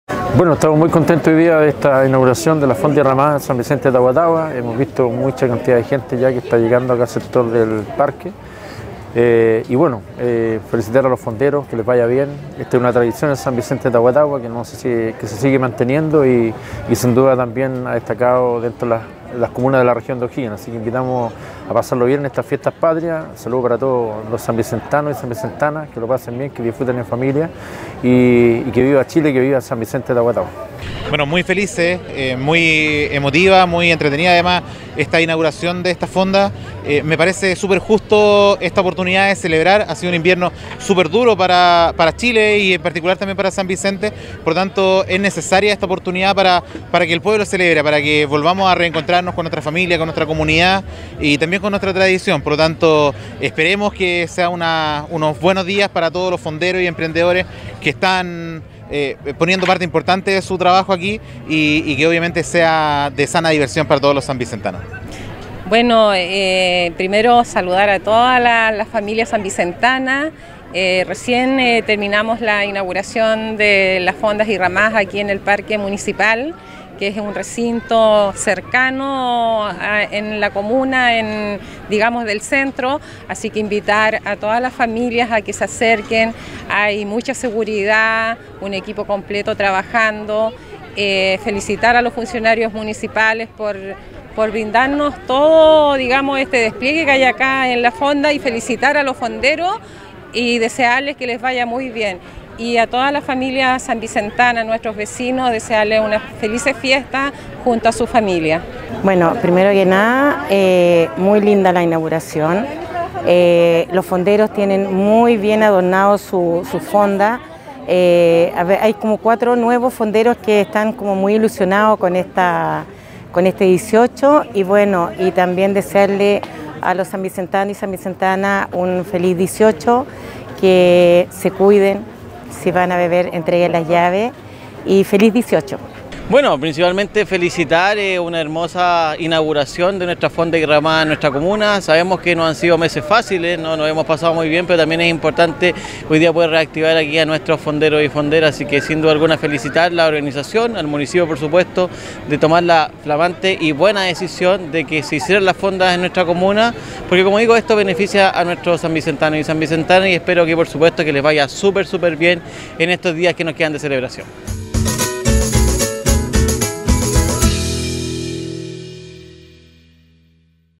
FIESTAS PATRIAS 2023
Concejales de nuestra comuna nos entregan su opinión respecto a la ceremonia de inauguración de Fondas y Ramadas, realizada este viernes 15 de septiembre en el parque municipal.